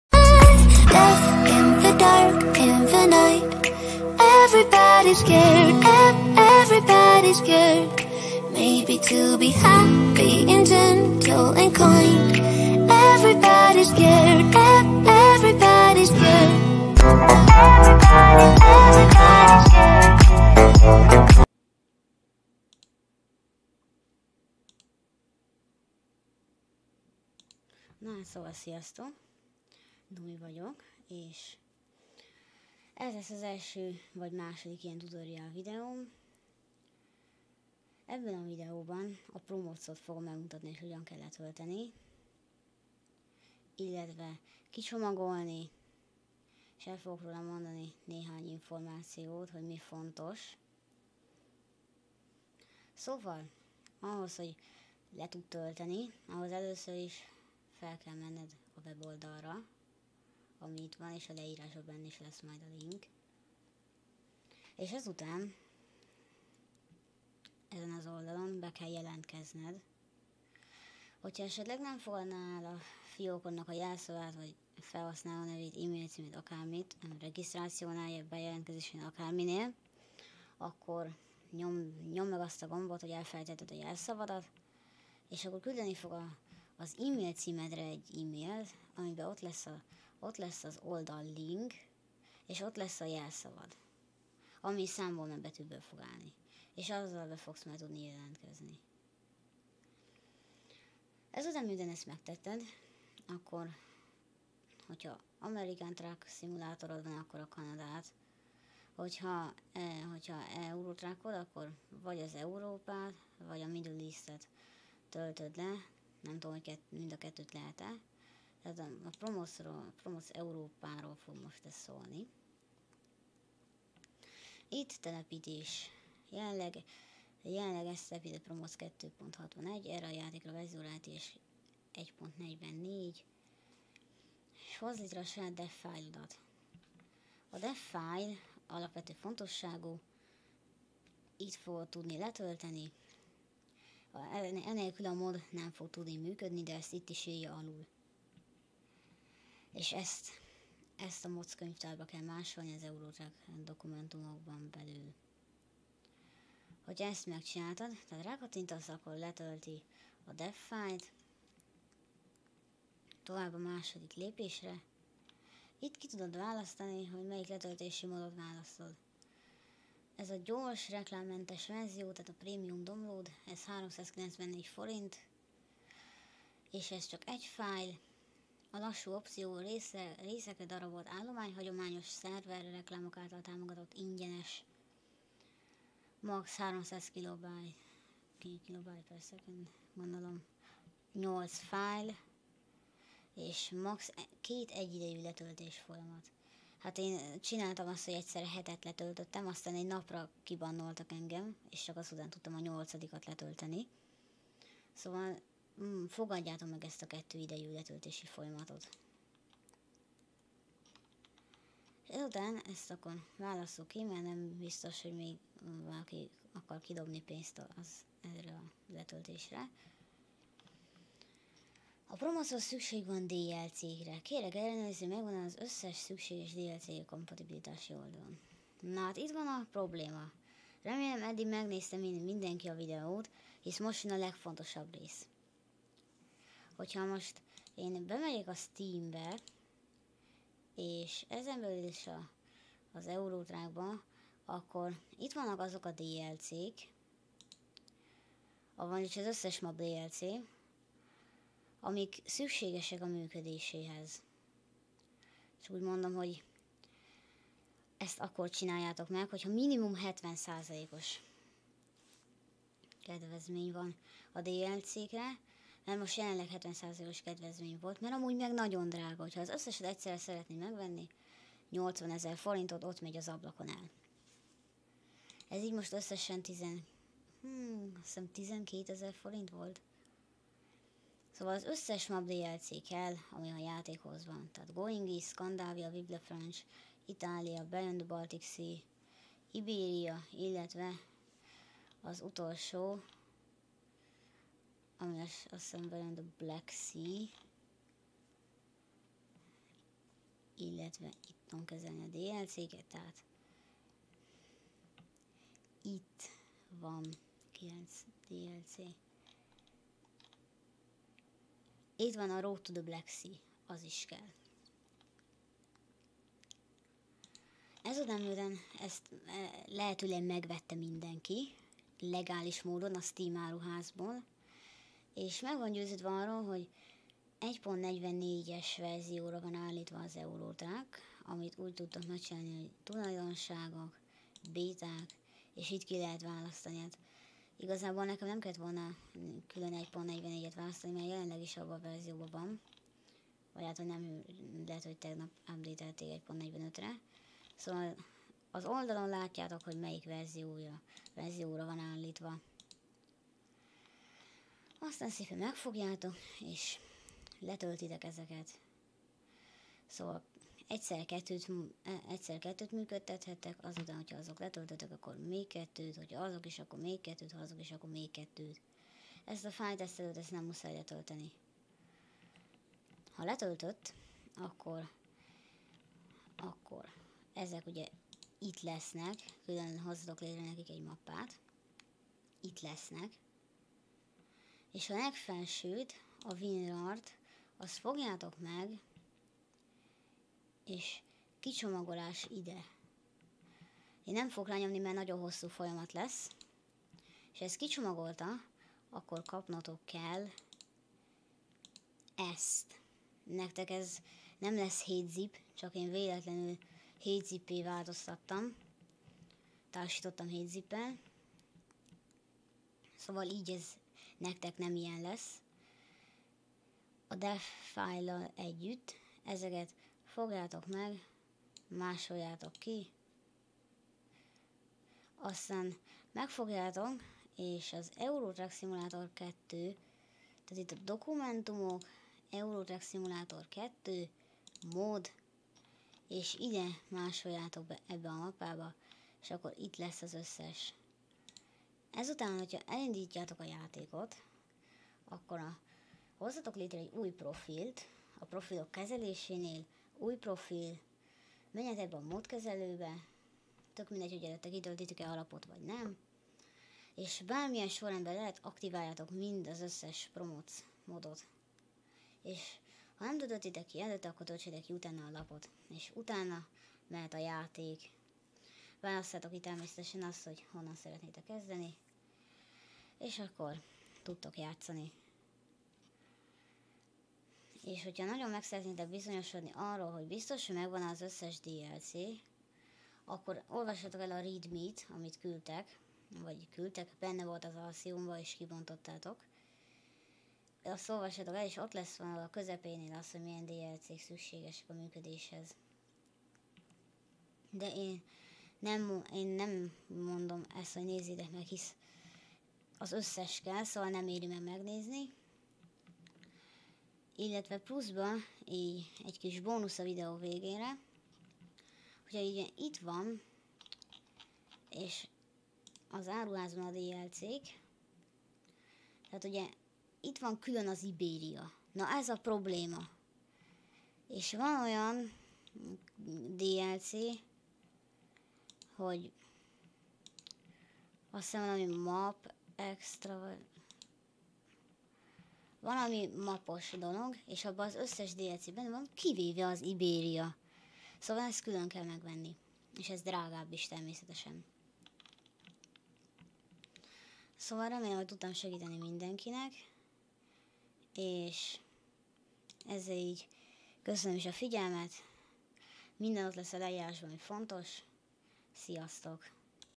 Promods 2.61 | ETS2 1.44-hez. Letöltése és kicsomagolása [TUTORIAL]
A Map Letöltéséhez szükséges az összes megvásárolható Map DLC!
A Steam ETS2 Map Csomag neve: Map Booster

Ez itt az új videó. 😉

Köszönöm ha végignézted a tartalmat!
Mindenféle véleményt elfogadok.

✏️Leírás utoljára módosítva: 2022/06/10✏️